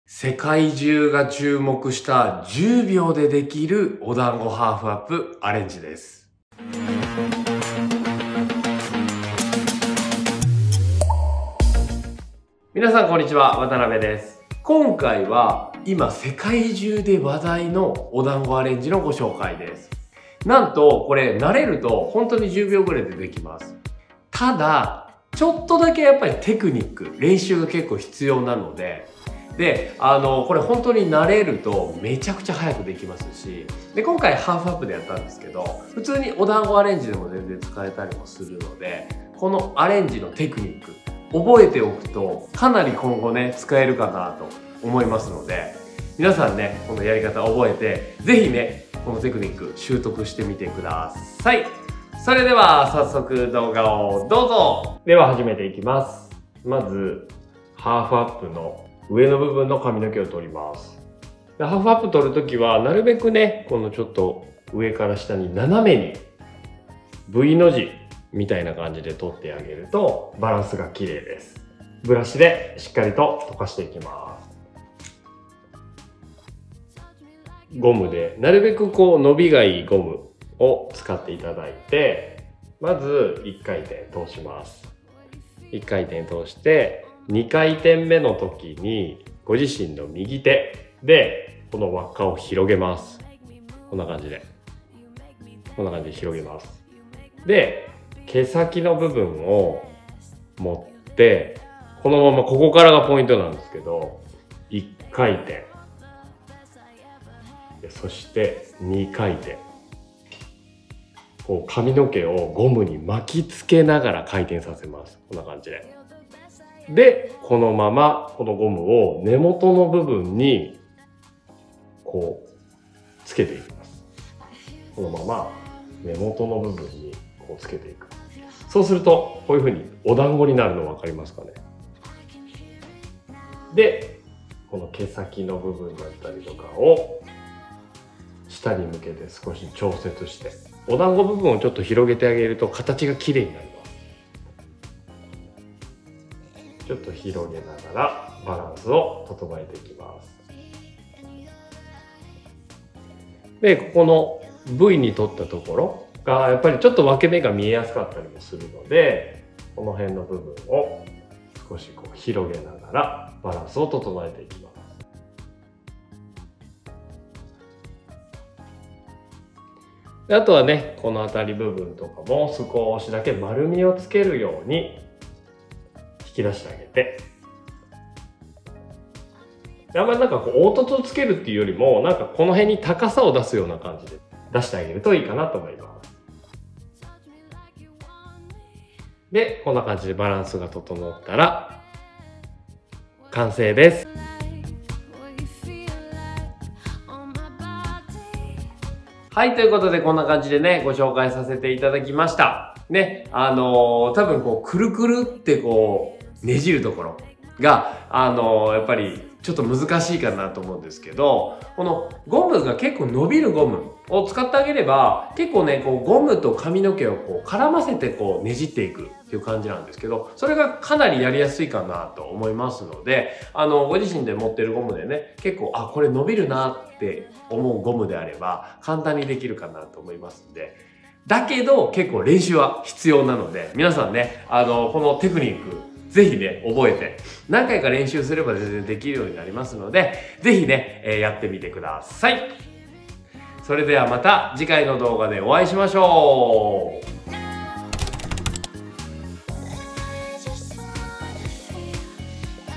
今、世界が注目している、10秒で作るお団子ヘアアレンジ 表参道美容師 SALONTube 渡邊義明
こんにちは

今回は今、世界中で話題の10秒で出来るお団子ヘアアレンジをご紹介です

正直、難しいのです笑
でも練習すれば必ず出来るようになりますので
ぜひやり方を覚えて練習してみてください。

※絶対によく伸びるゴムでやってくださいね！！

ちなみに僕もこの動画で何度もやり直しました笑

Hair styling
Hair arrangement
头发
헤어